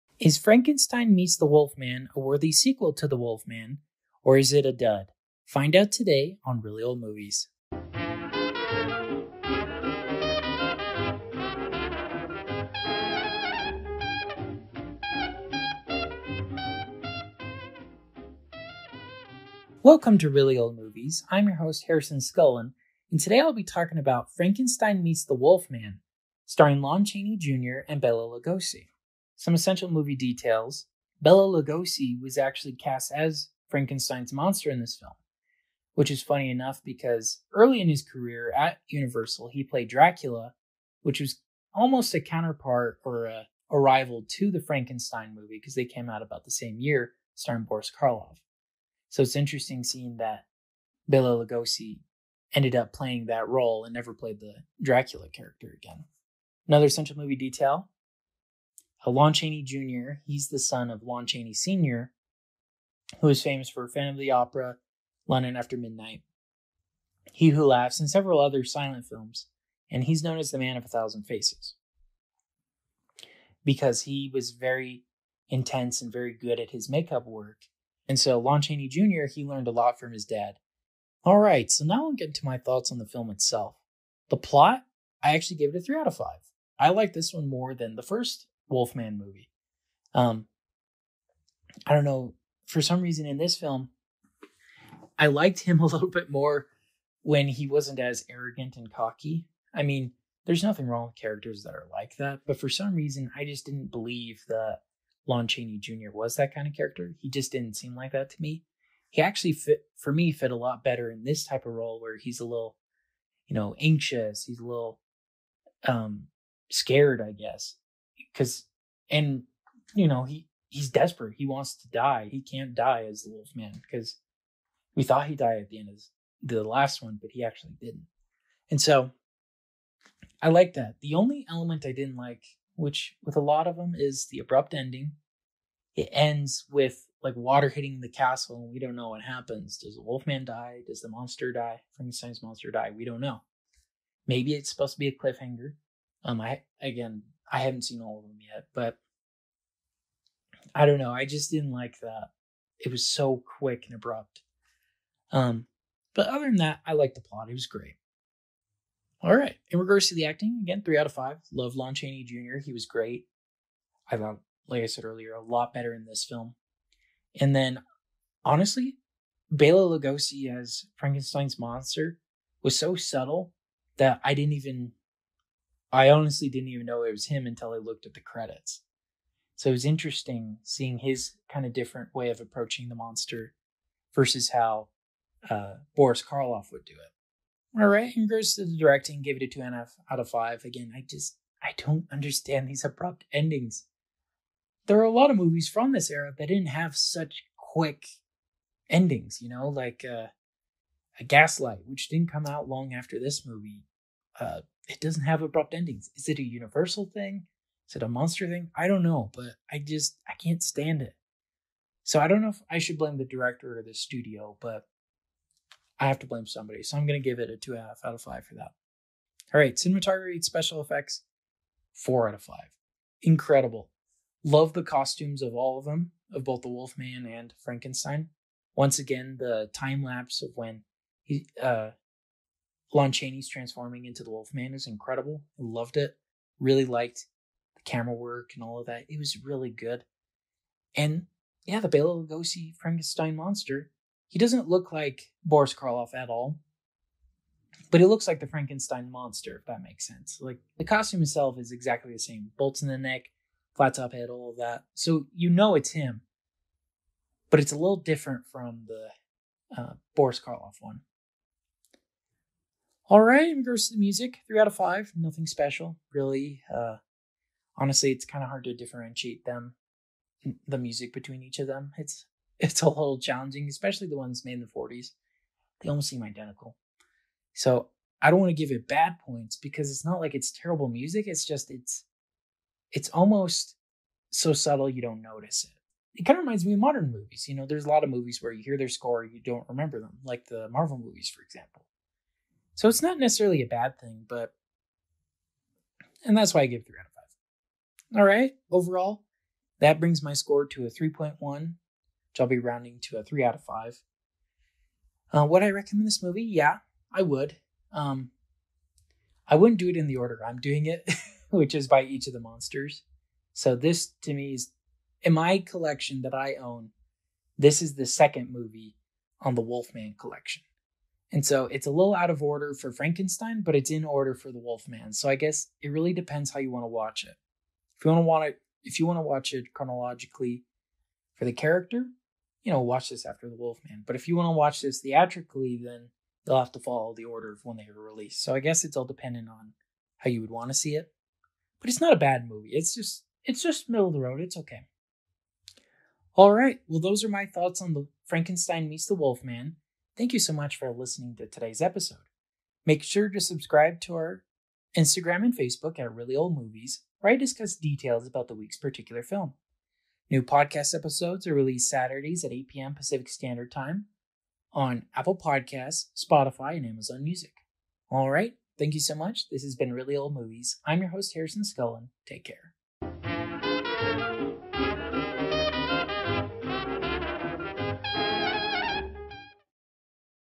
#59 "Frankenstein Meets The Wolf-Man (1943)" HALLOWEEN 2022 REVIEW (10/15/22)
Continuing in our Halloween Reviews,  Harrison will be reviewing "Frankenstein Meets The Wolf-Man (1943)"